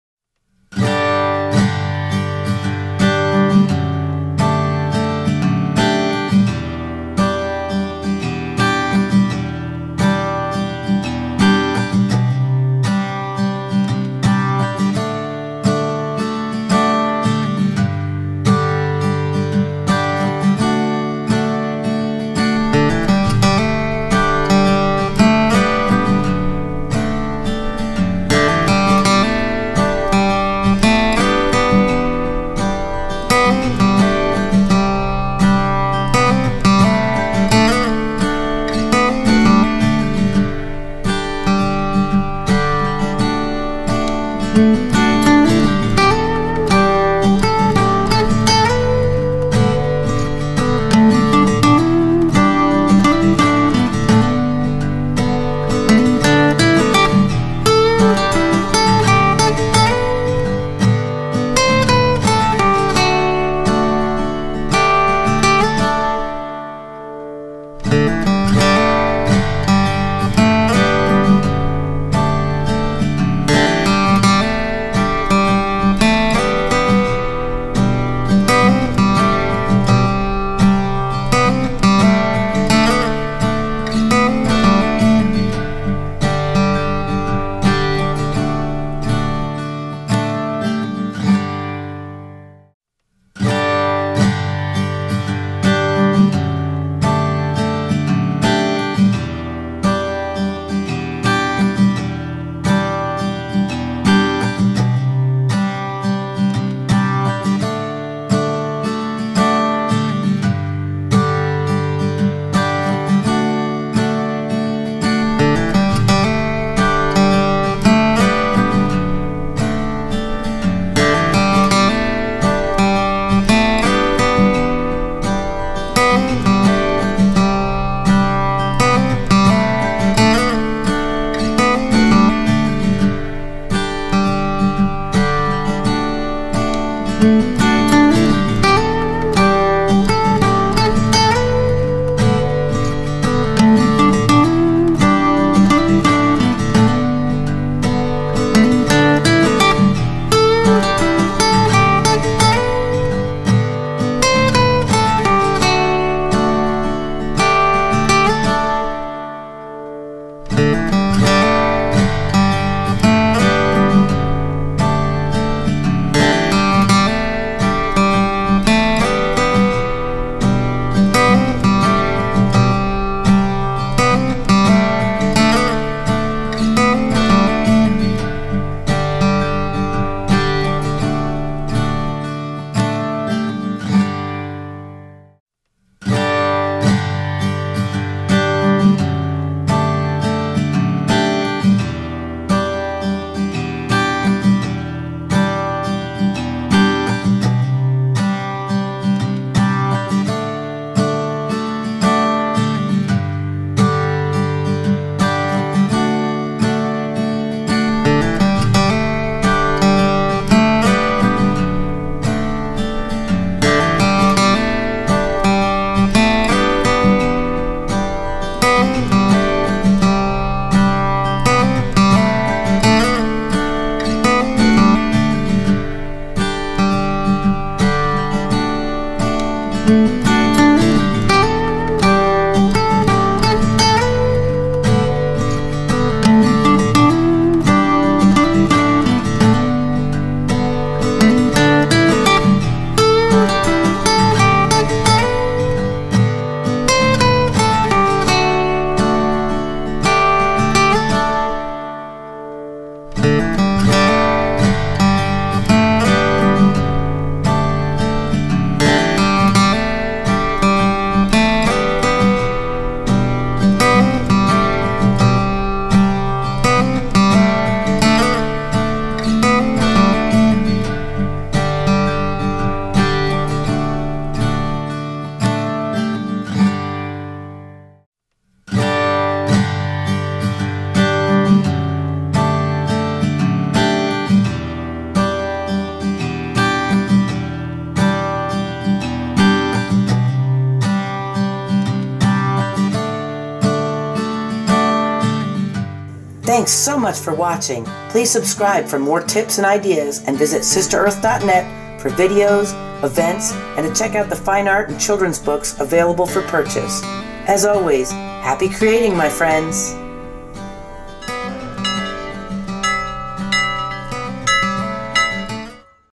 Explore Paint Pour! Spin Table Tree Ring Pours!
Taking the tree ring pour technique to the next level with a spin table!  And I couldn't stop at just one!